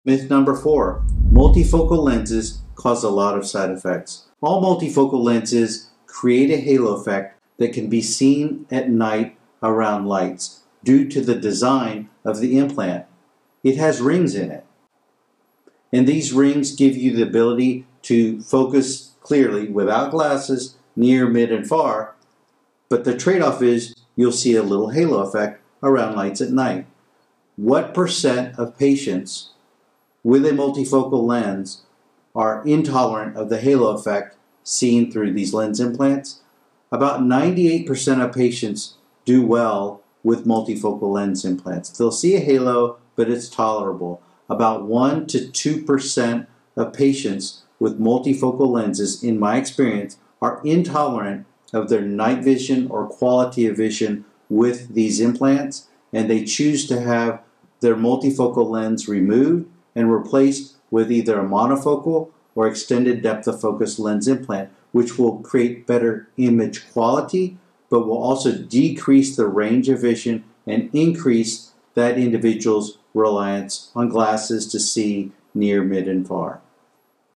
Premium Lens implants. Do multifocal lens implants have a lot of side effects?
a. All multifocal lenses create a halo effect around lights that can be seen at night due to the design of the multifocal implant…it has rings in the implant that create the halo
b. How often are these halos intolerable?  For most current generation multifocal lens implants, 98% of patients learn to adapt to the halo effect and see well. 1-2% of patients with multifocal lenses in my experience will not tolerate the halos and will ultimately have their multifocal lenses removed and replaced with a monofocal or extended depth of focus lens that provides high quality vision with greater reliance on glasses than a multifocal lens (“there is no perfect lens”)